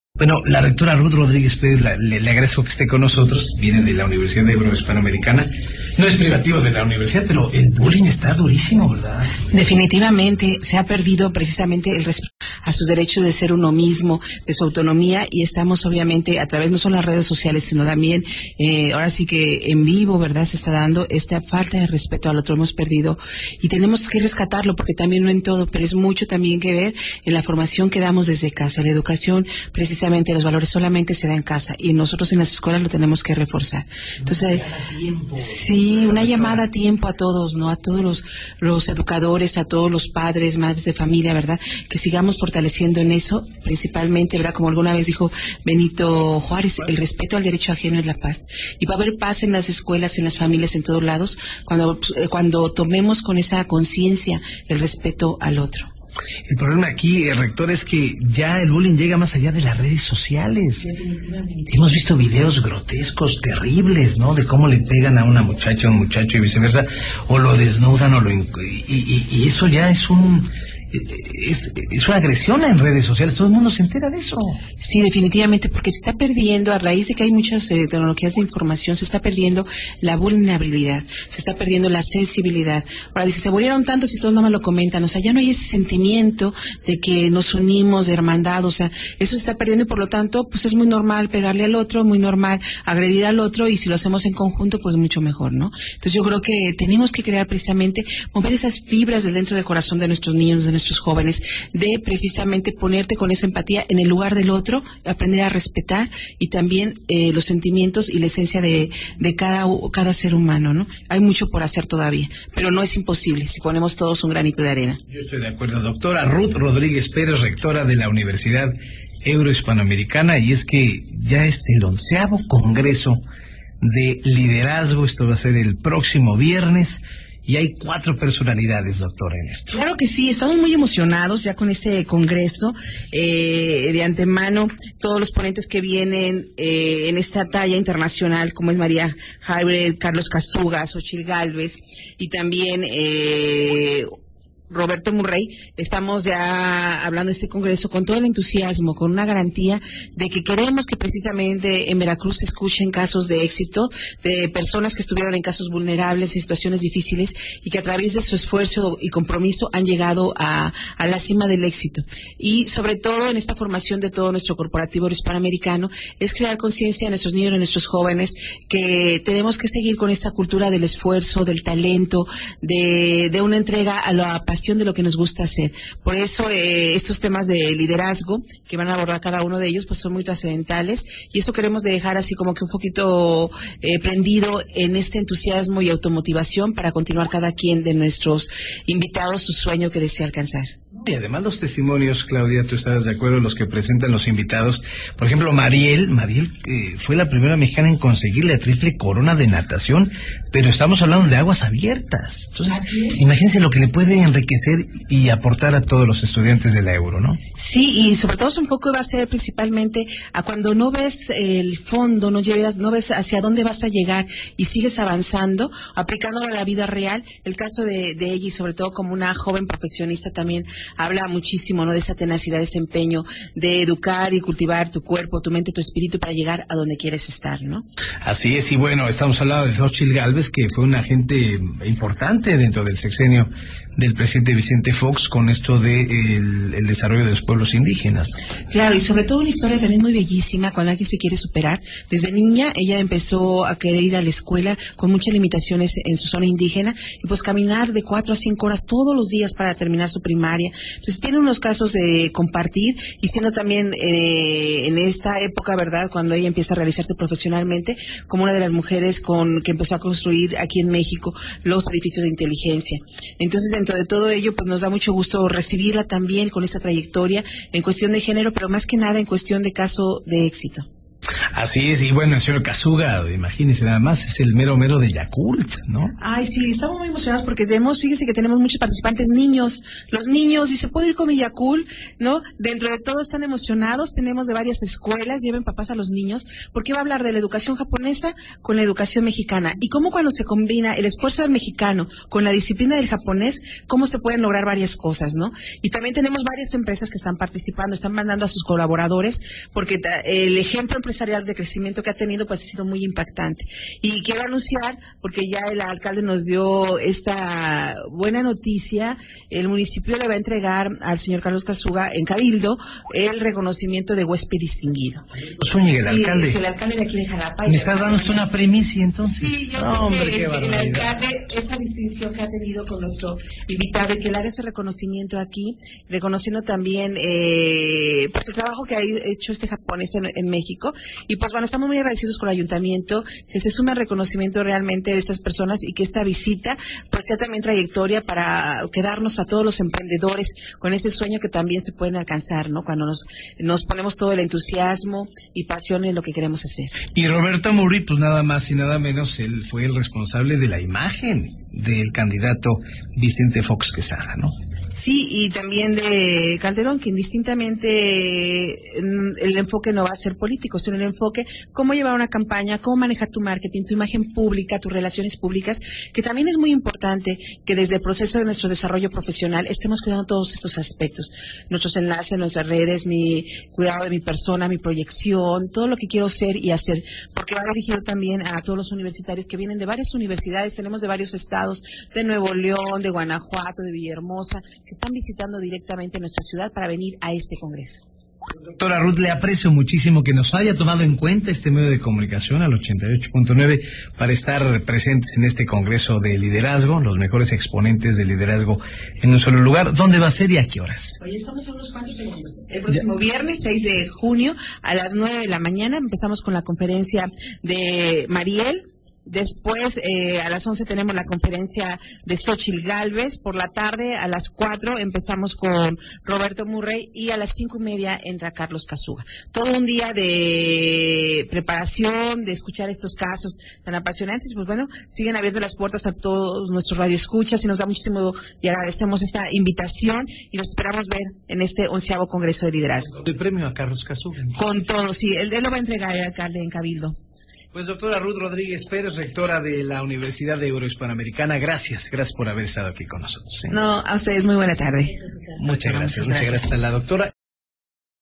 Uni Noticias 88.9 fm: Entrevista a Ruth María Rodríguez Pérez
Entrevista realizada por el periodista Ramsés Yunes Zorrilla a Ruth María Rodriguez Pérez, rectora de la Universidad Euro Hispanoamericana, con motivo del 11° Congreso de Liderazgo a realizarse este próximo 6 de junio de 2014 (03/06/2014).